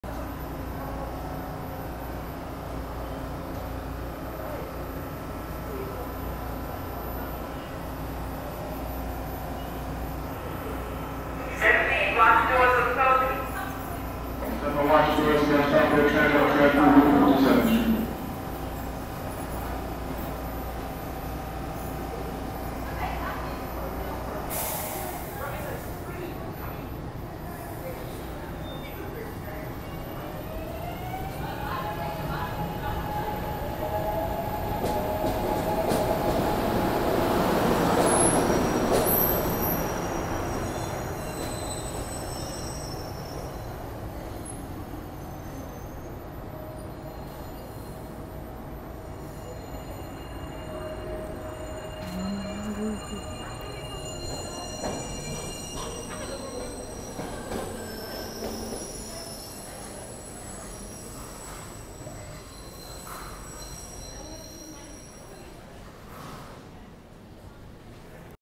Septa market Frankford Line and septa subway service announcement at 69th street terminal
When I arrived at 69th Street Transportation Center when I got off the el train after I was expressed from 40th street station to 69th Street Transportation Center on the westbound el train and all of the sudden the el train was going to be express to 52nd Street station from 69th Street Transportation Center and I also heard the el train announcement and the PA announcement on the platform at 69th Street.